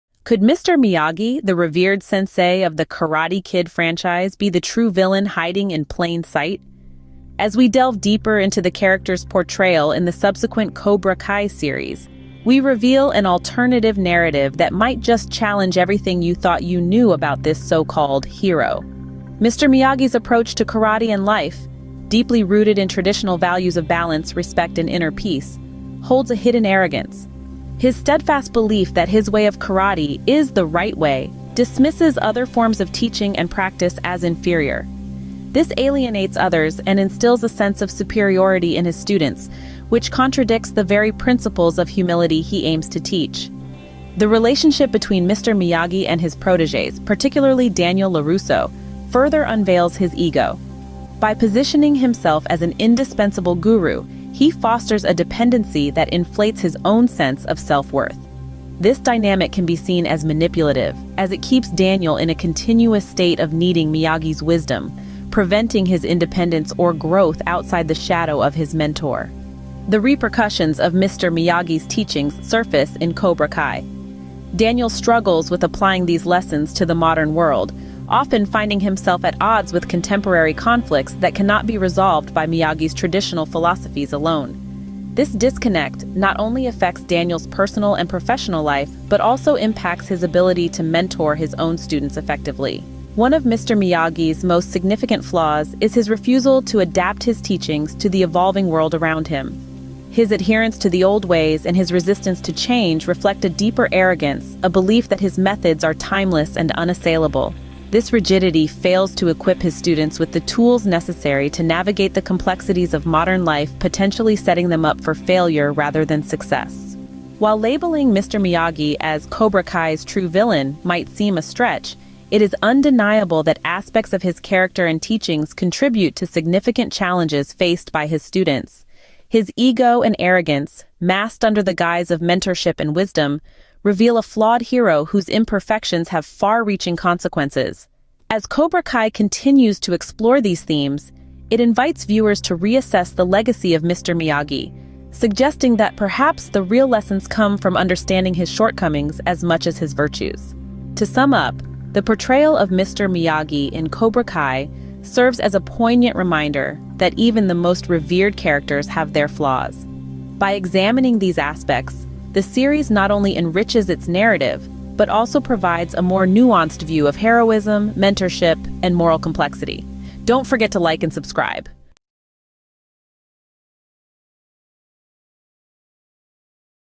Arrogant Side of Mr. Miyagi Exposed in Cobra Kai
Uncover the truth about Mr. Miyagi's ego and arrogance in Cobra Kai. Dive deep into the Karate Kid secrets and learn about the true villain of the show in this analysis.

Mr. Miyagi: Cobra Kai’s Hidden Villain? Exploring His Ego & Arrogance
🥋👀 Dive deep into the "Karate Kid" saga with our latest video, where we explore a bold new perspective on Mr. Miyagi in "Cobra Kai." Known for his wisdom and peaceful approach, Mr. Miyagi has always been seen as the virtuous mentor. But what if there's more to him than meets the eye? Could his ego and arrogance subtly make him the true villain of the series?

🔍 In this video, we uncover the hidden layers of Mr. Miyagi's character:

The Arrogance Unseen: Is Mr. Miyagi's insistence on his style of karate fostering a sense of superiority?
Dependency Dynamics: Analyze how his mentorship might actually hinder Daniel LaRusso’s independence.
Philosophical Fallout: See how his traditional methods clash with modern challenges in "Cobra Kai."
Adaptation and Arrogance: Could his refusal to evolve with the times be considered arrogance?
🎬 Join us as we challenge the traditional view of Mr. Miyagi, suggesting his imperfections might have far-reaching impacts. Is he setting up his students for failure in a world he refuses to accept has changed?

👊💥 Watch now and decide for yourself – is Mr. Miyagi really the hero we all believed him to be, or is there a darker truth to uncover?